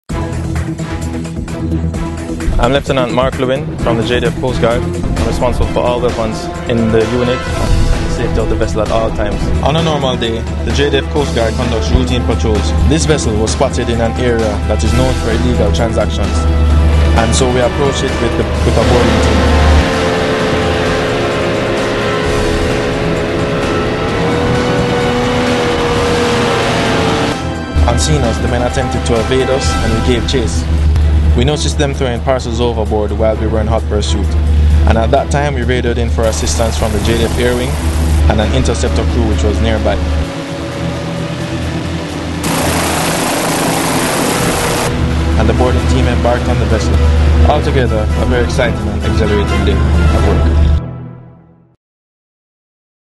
The Jamaica Defence Force Coast Guard
A typical day as a member of the JDF Coast Guard